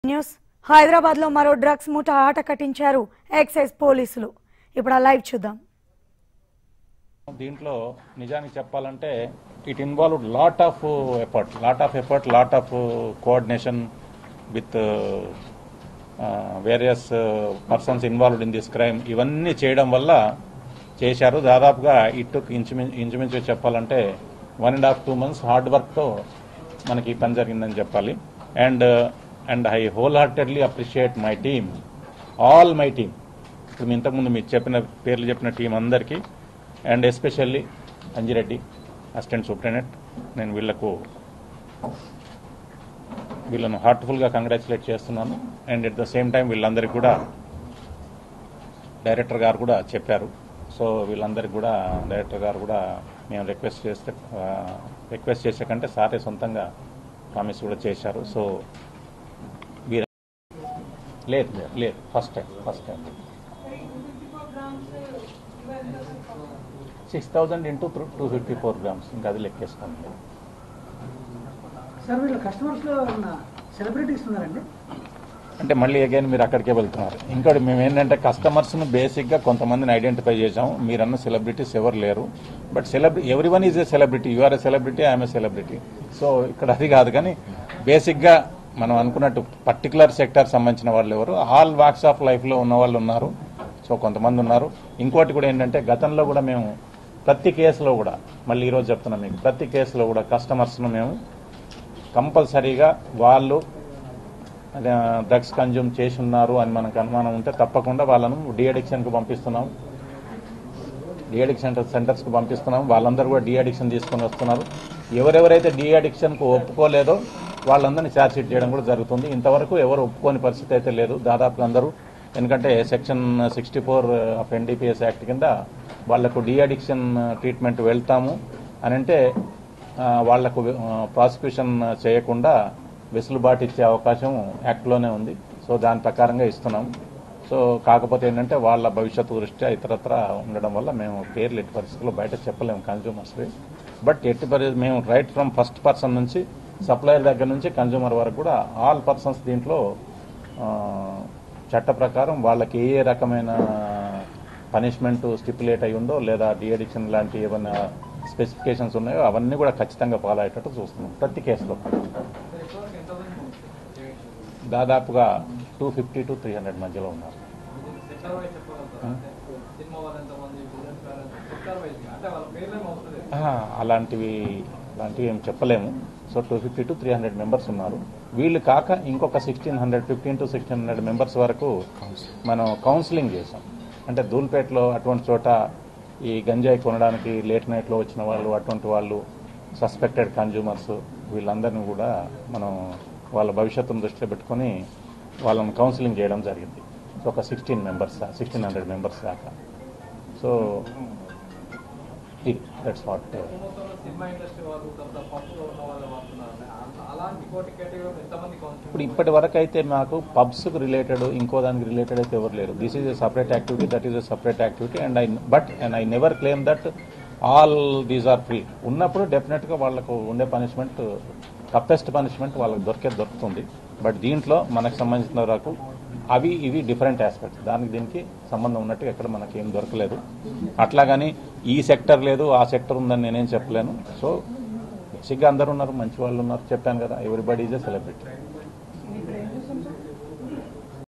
Excise Police Bust Nigerian Drugs Gang in Golconda, Hyderabad | TV5 News
Excise Police Busts Nigerian Drugs Gang in Golconda, Hyderabad